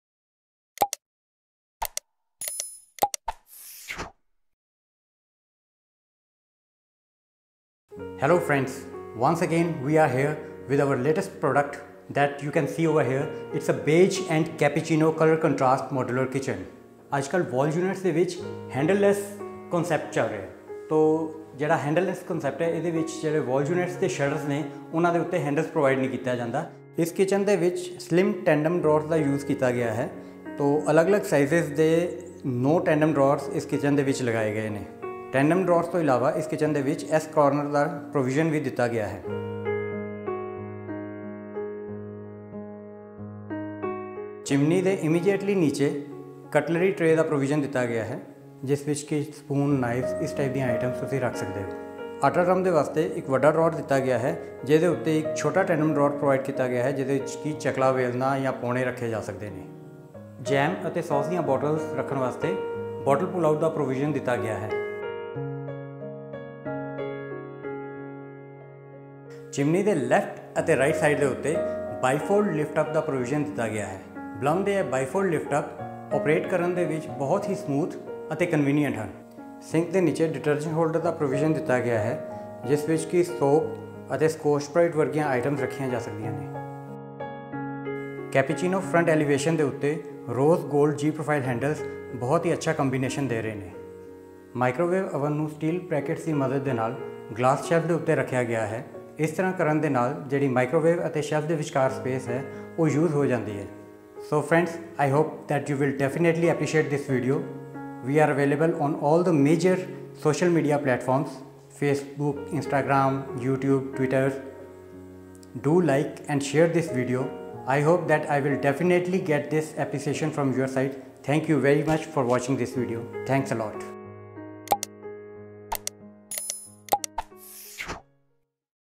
Modular Kitchen with handless shutters on wall unit
This Modular Kitchen is made up of Action Tesa (Boilo) material.
Boilo is Termite resistant, water resistant and fire retardant as well.
Front acrylic elevation is of acrylic shutters with an eye catching colour contrast. Aluminium profile handles used in this kitchen are trendy these days.
We do make Aluminium Kitchens, Modular Kitchens, Almirahs, Sliding door wardrobes, LED pannels, Vanities and do other miscellaneous interior works.

|Modular Kitchen | Boilo | Metals & Interiors |

Hi!